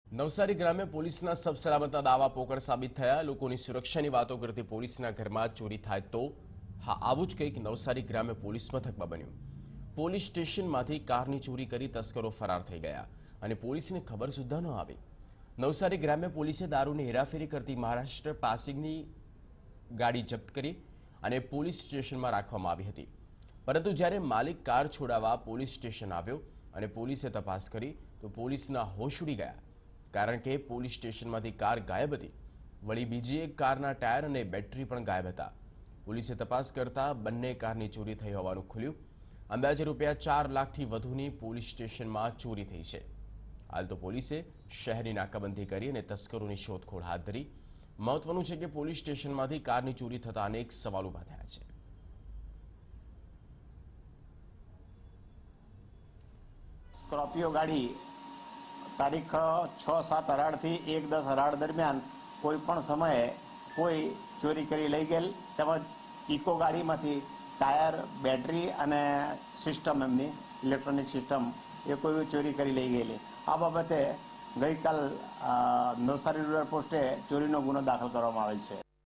Seized car stolen from police station , Navsari - Tv9
Seized car stolen from police station , Navsari.